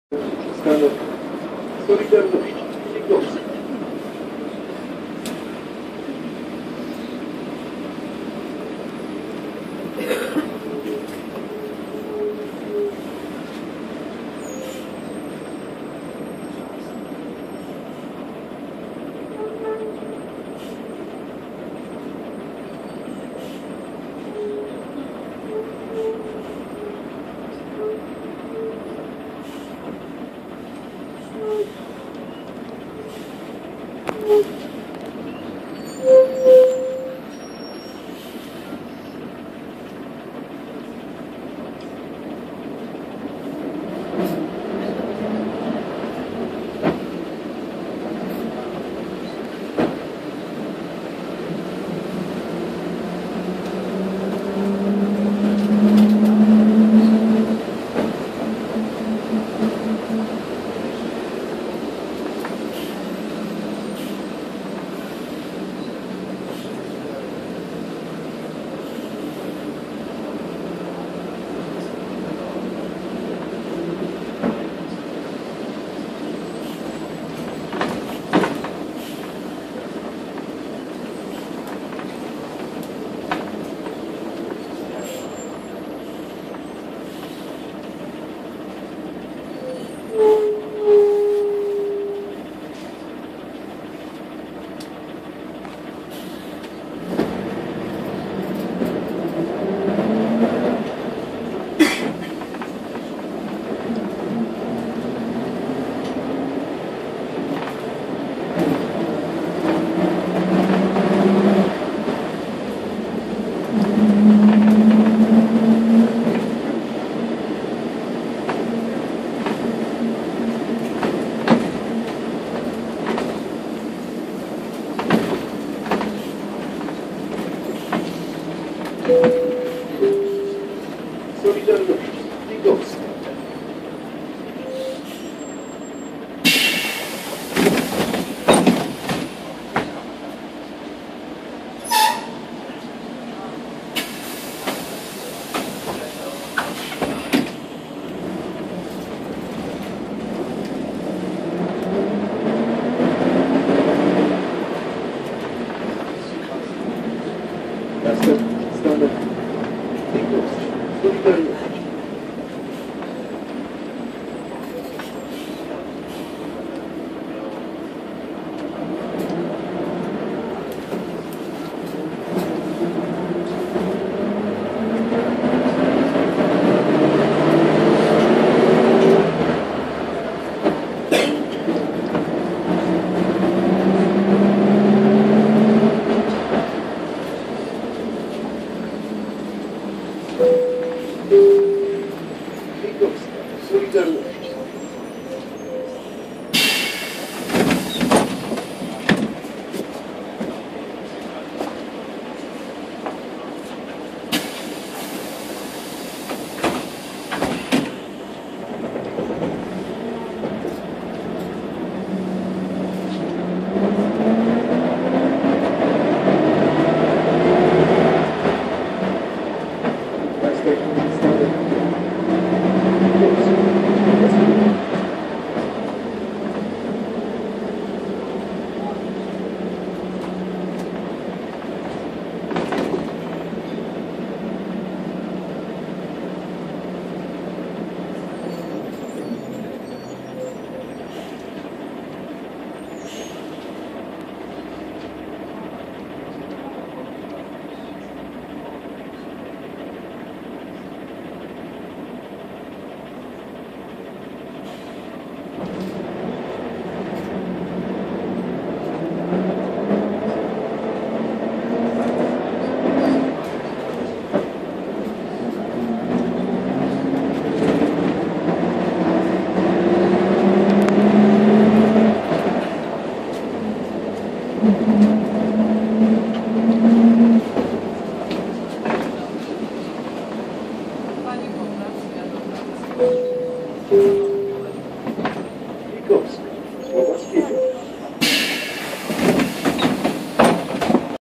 Jelcz PR110 CNG #173
:) Przejazd gazowym jelczem#173 na linii 3 w Mpk Tarnów :) . Kurs w kierunku pętli "Biała Górki " :) .